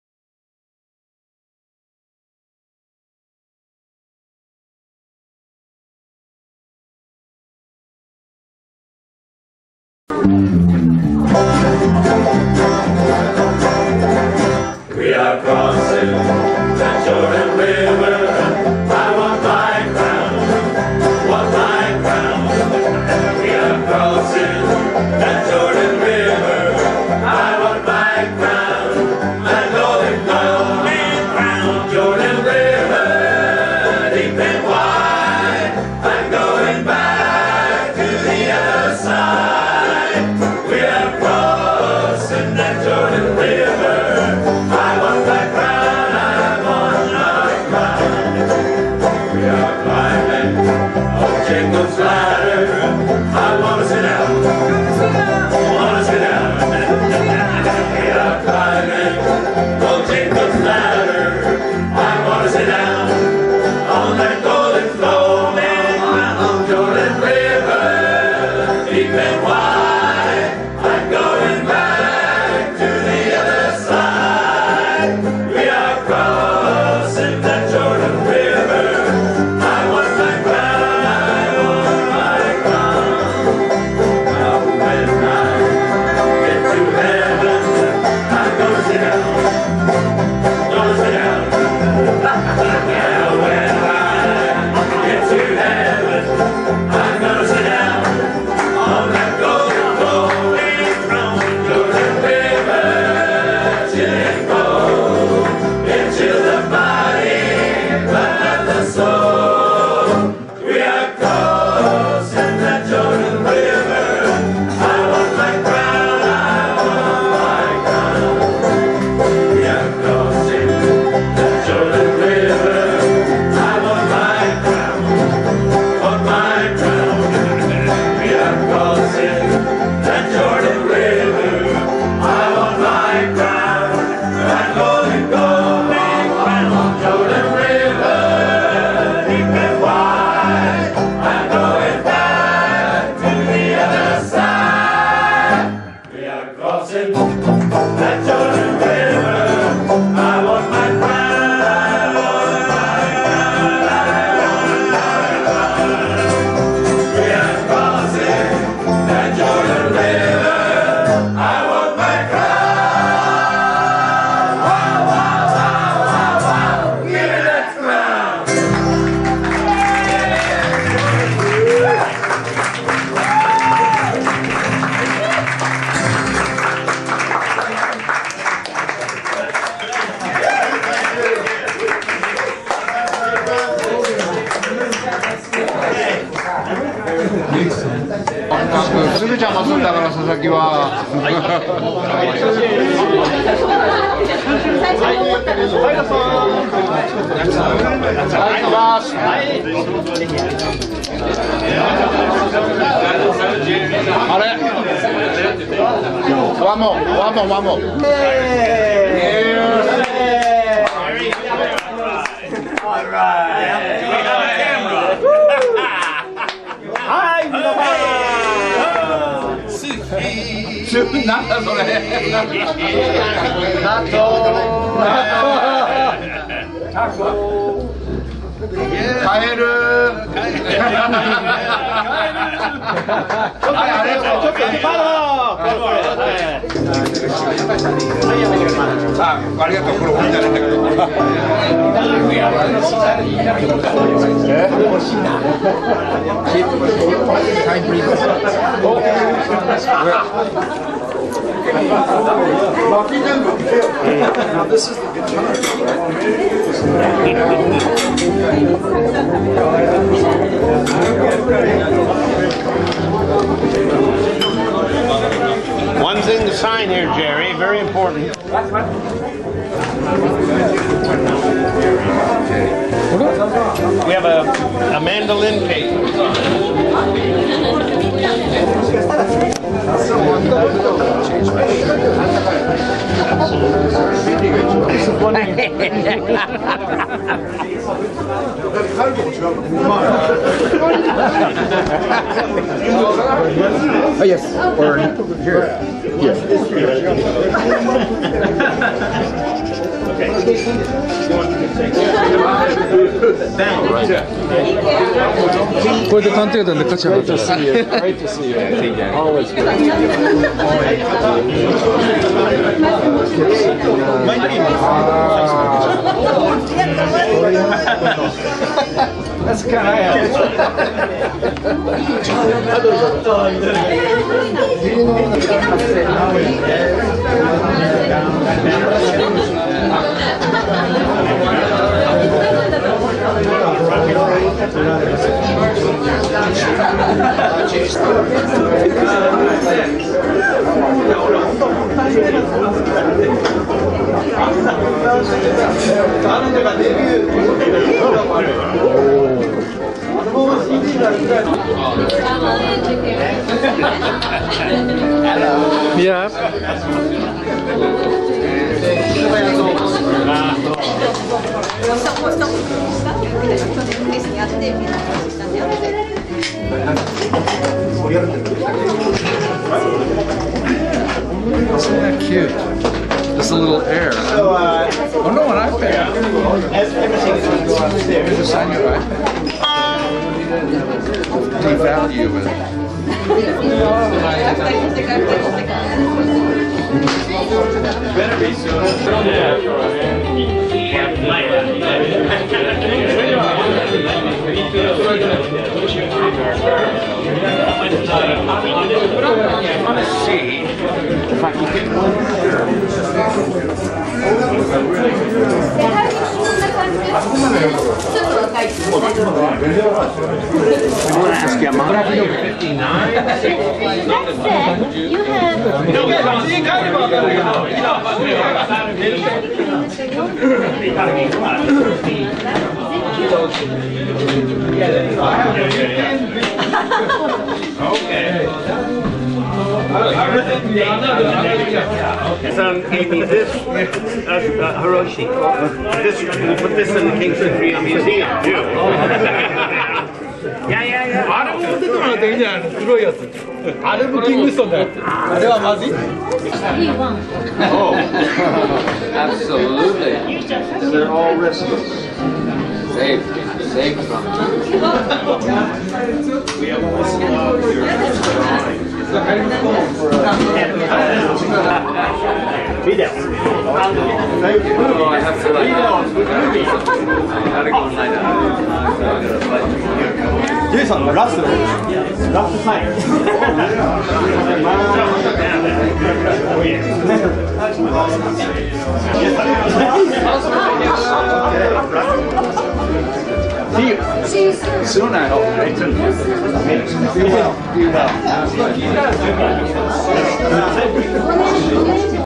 2011107 MFQ with fans in Tokyo, Japan
２０１１年１１月７日
Ｍｏｄｅｒｎ　Ｆｏｌｋ　Ｑｕａｒｔｅｔ　Ｊａｐａｎ,　Ｔｏｕｒ　２０１１
Ｆａｒｅｗｅｌｌ　Ｐａｒｔｙ